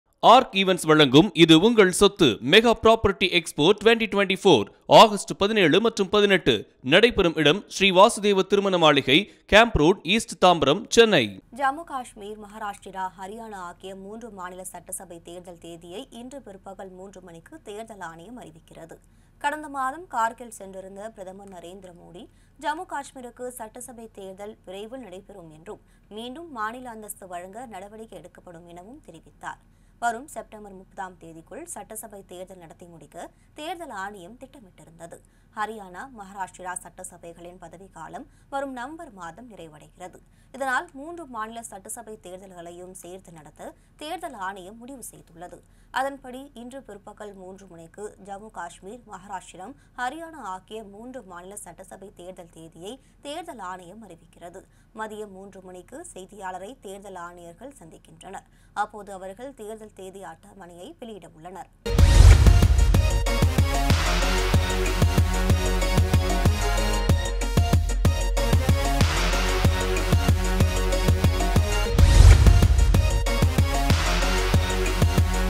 3 மாநிலத் தேர்தல்... இன்று அறிவிக்கிறது தேர்தல் ஆணையம்! | Election | Announcement
ஜம்மு காஷ்மீர், மஹாராஷ்டிரா, ஹரியானா ஆகிய 3 மாநில சட்டசபை தேர்தல் தேதியை இன்று பிற்பகல் 3 மணிக்கு தேர்தல் ஆணையம் அறிவிக்கிறது.